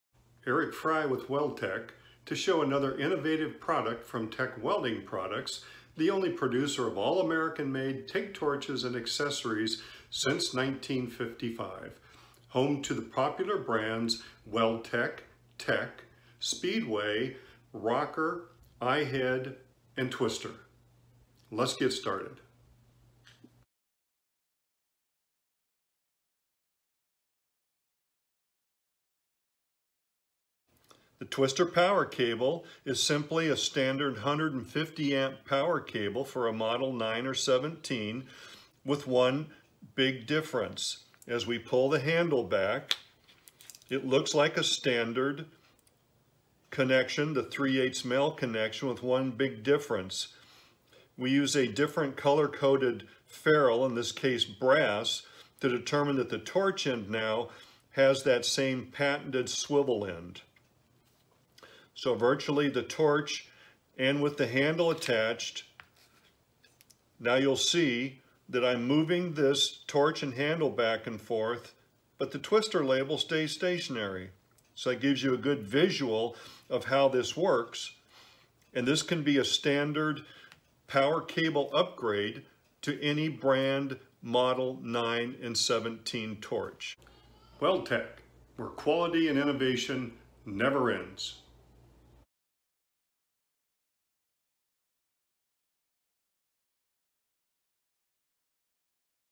Weldtec TIG Welding Products (Twister Power Cable)
Patented swivel power connection for 150A & 200A 1pc. TIG power cables allowing the model 9 or 17 torch and handle to swivel over the power cable,